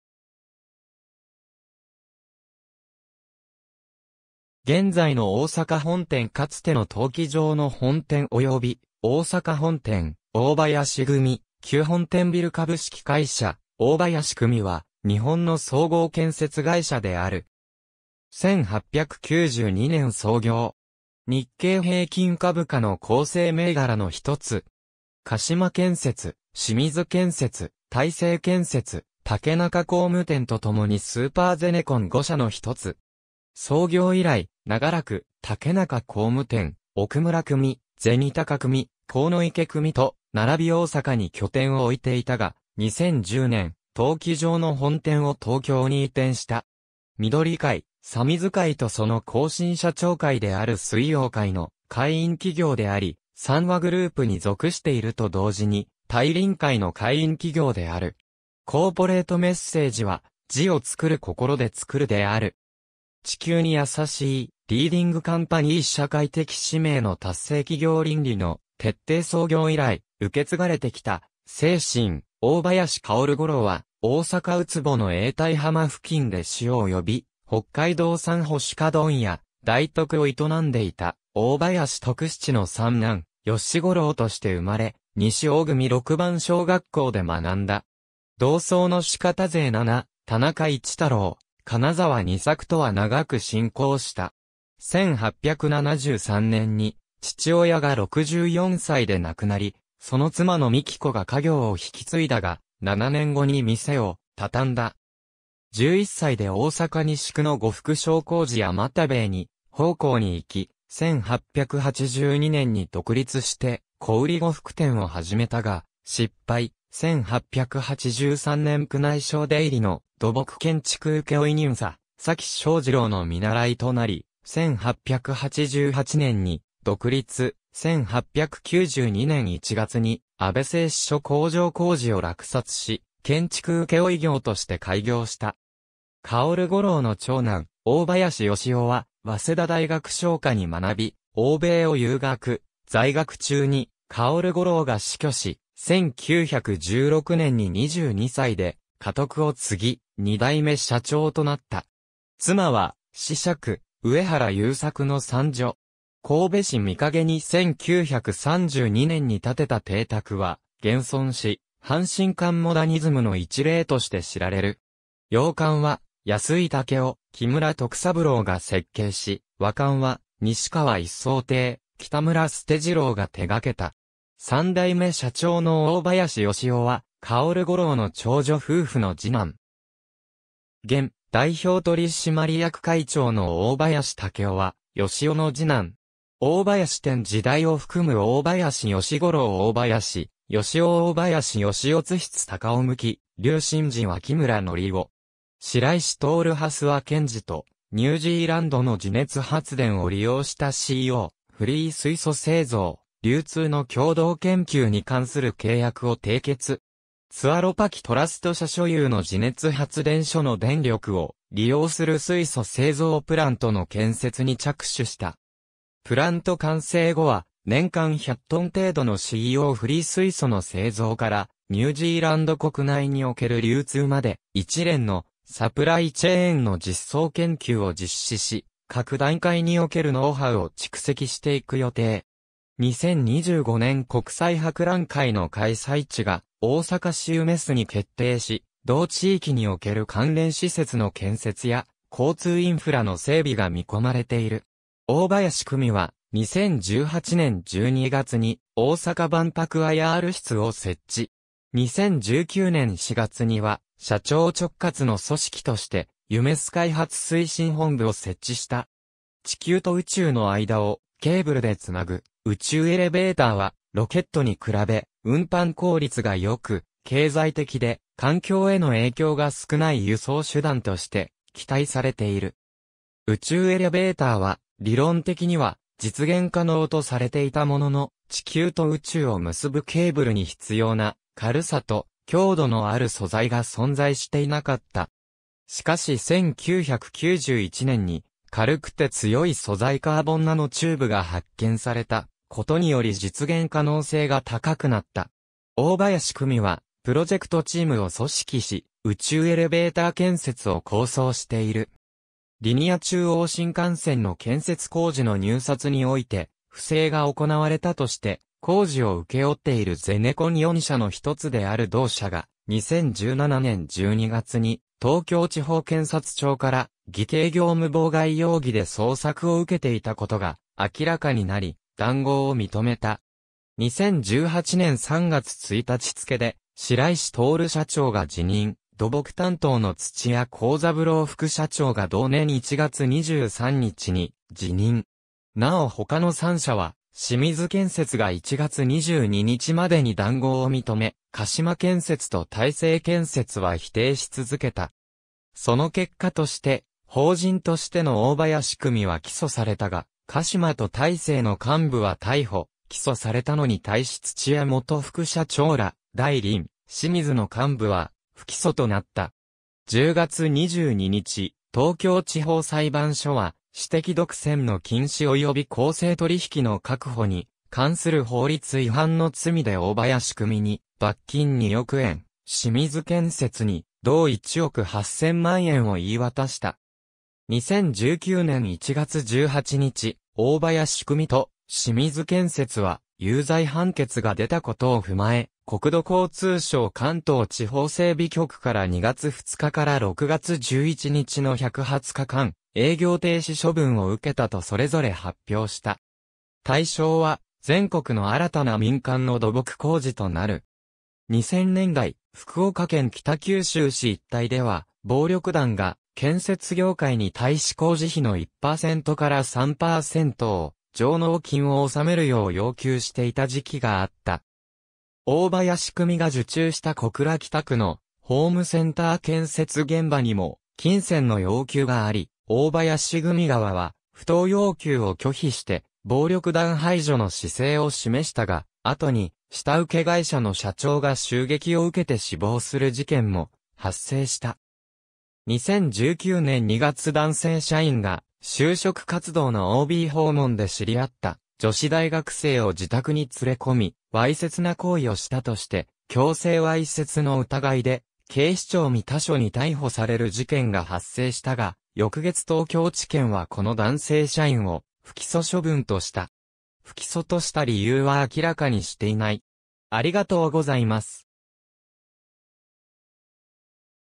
大林組
現在の大阪本店（ダイビル本館、大阪市北区） かつての登記上の本店および大阪本店（大阪大林ビルディング、大阪市中央区） 大林組旧本店ビル（ルポンドシエル ビル、大阪市中央区） 株式会社大林組（おおばやしぐみ）は、日本の総合建設会社である。
1892年創業。
日経平均株価の構成銘柄の一つ。
鹿島建設、清水建設、大成建設、竹中工務店とともにスーパーゼネコン5社の一つ。
創業以来、永らく竹中工務店、奥村組、錢高組、鴻池組と並び大阪に拠点を置いていたが、2010年（平成22年）、登記上の本店を東京に移転した。
みどり会・三水会とその後身社長会である水曜会の会員企業であり三和グループに属していると同時に大輪会の会員企業である。
コーポレートメッセージは「時をつくる こころで創る」である。
「地球に優しい」リーディングカンパニー 社会的使命の達成 企業倫理の徹底 創業以来、受け継がれてきた精神 大林芳五郎（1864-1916）は、大阪靱の永代浜付近で塩及び北海道産干鰯問屋「大徳」を営んでいた大林徳七の三男・由五郎として生まれ、西大組六番小学校で学んだ。
同窓の志方勢七、田中市太郎（田中市兵衛長男）、金沢仁作とは長く親交した。
1873年に父親が64歳で亡くなり、その妻の美喜子が家業を引き継いだが、7年後に店をたたんだ。
11歳で大阪西区の呉服商麹屋又兵衛に奉公にいき、1882年に独立して小売呉服店を始めたが失敗、1883年宮内省出入りの土木建築請負人砂崎庄次郎の見習いとなり、1888年に独立、1892年1月に阿部製紙所工場工事を落札し、建築請負業として開業した。
芳五郎の長男・大林義雄 (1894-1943)は、早稲田大学商科に学び、欧米を遊学、在学中に芳五郎が死去し、1916年に22歳で家督を継ぎ、2代目社長となった。
妻は子爵上原勇作の三女。
神戸市御影に1932年に建てた邸宅は現存し（現・大林組迎賓館 北緯34度43分32秒 東経135度15分02秒 / 北緯34.72557度 東経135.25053度 / 34.72557; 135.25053）、阪神間モダニズムの一例として知られる。
洋館は安井武雄、木村得三郎が設計し、和館は西川一草亭（津田青楓の兄）、北村捨次郎が手掛けた。
三代目社長の大林芳郎は芳五郎の長女夫婦の二男。
現・代表取締役会長の大林剛郎は芳郎の二男。
※「大林店」時代を含む 大林芳五郎（1892年 - 1916年） 大林義雄（1916年 - 1943年） 大林芳郎（1943年 - 1989年） 津室隆夫（1989年 - 1997年） 向笠愼二（1997年 - 2005年） 脇村典夫（2005年 - 2007年）
白石達 （2007年 - 2018年） 蓮輪賢治（2018年 - ) 2018年11月、ニュージーランドのTuaropaki Trust（トゥアロパキ・トラスト）と「ニュージーランドの地熱発電を利用したＣＯ₂フリー水素製造・流通の共同研究」に関する契約を締結。
Tuaropaki Trust社所有の地熱発電所の電力を利用する水素製造プラントの建設に着手した。
プラント完成（2020年予定）後は、年間100ｔ程度のＣＯ₂フリー水素の製造からニュージーランド国内における流通まで、一連のサプライチェーンの実装研究を実施し、各段階におけるノウハウを蓄積していく予定。
2025年国際博覧会（万博）の開催地が大阪市夢洲に決定し、同地域における関連施設の建設や交通インフラの整備が見込まれている。
大林組は、2018年12月に「大阪万博ＩＲ室」（現：「大阪関西万博・ＩＲ室」）を設置。
2019年4月には、社長直轄の組織として「夢洲開発推進本部」を設置した。
地球と宇宙の間をケーブルでつなぐ「宇宙エレベーター」は、ロケットに比べ、運搬効率が良く、経済的で、環境への影響が少ない輸送手段として期待されている。
「宇宙エレベーター」は、理論的には実現可能とされていたものの、地球と宇宙を結ぶケーブルに必要な「軽さ」と「強度」のある素材が存在していなかった。
しかし1991年に、軽くて強い素材カーボンナノチューブが発見されたことにより実現可能性が高くなった。
大林組はプロジェクトチームを組織し、宇宙エレベーター建設を構想している。
リニア中央新幹線の建設工事の入札に於いて不正が行われたとして、工...